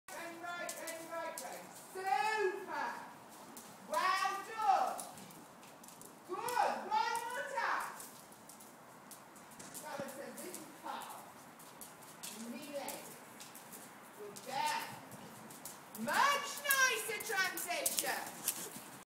Salazar flatwork lesson
Salazar is 21yo and is my flatwork schoolmaster!  I am learning a simple change through trot.